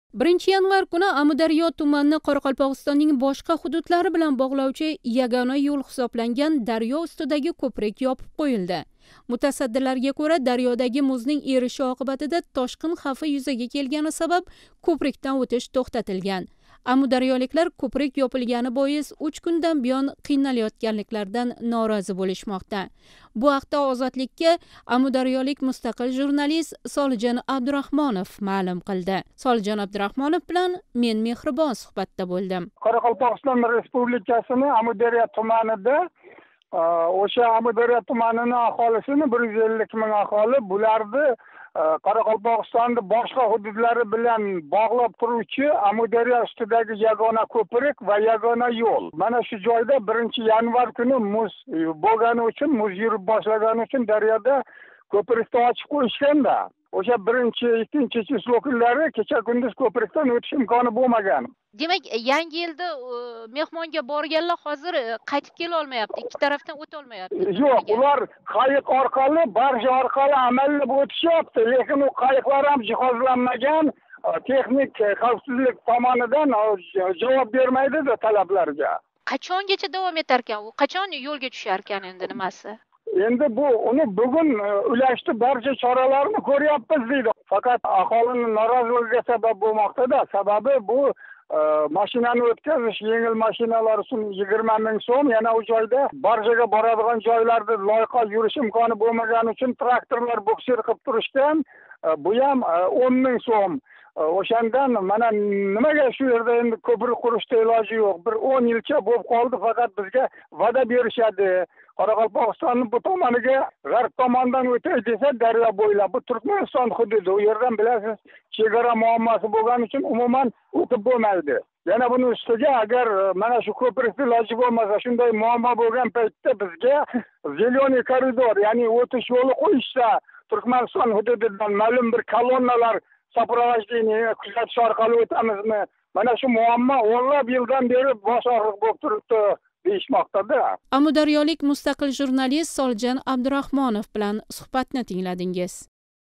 Тошқин хавфи боис Амударё кўприги ёпиб қўйилди
1 январь куни Амударё туманини Қорақалпоғистоннинг бошқа ҳудудлари билан боғловчи ягона кўприк ёпиб қўйилди. Мутасаддиларга кўра, Амударёдаги музнинг эриши оқибатида тошқин хавфи юзага келгани сабаб кўприкдан ўтиш тўхтатилган. Амударёликлар кўприк ёпилгани боис уч кундан буён қийналаётганликларидан норози бўлмоқда.
Амударё туманида яшовчи мустақил журналист Солижон Абдураҳмоновнинг Озодликка хабар беришича, Амударё устидаги кўприк уч кундан буён ёпиқ қолаётгани сабаб одамлар норози бўлмоқда:

- Амударё туманини Қорақалпоғистоннинг бошқа туманлари билан боғлаб турувчи Амударё устидаги ягона кўприк ва ягона йўл 1 январь куни дарё музи юра бошлагани учун ёпиб қўйилди. 1-, 2- январь кунлари кўприкдан ўтиш имкони бўлмаган. Янги йилда меҳмонга кетганлар қайиқда амаллаб ўтишаяпти. Лекин қайиқлар ҳам жиҳозланмаган, техник хавфсизлик томонидан жавоб бермайди,- деди Абдураҳмонов.